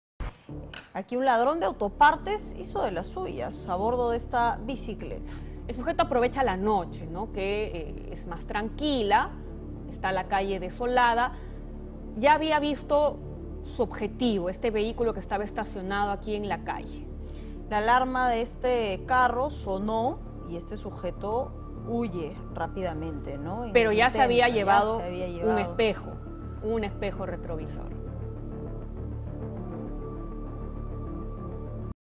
Surquillo: Ladrón usa bicicleta para robar autopartes
En el distrito de Surquillo, ladrón de autopartes hizo de las suyas a bordo de una bicicleta.

El sujeto aprovechó la noche y la calle desolada.

Dejó su bicicleta a un lado y arrancó el espejo.

La alarma de la unidad sonó y el sujeto huyó rápidamente.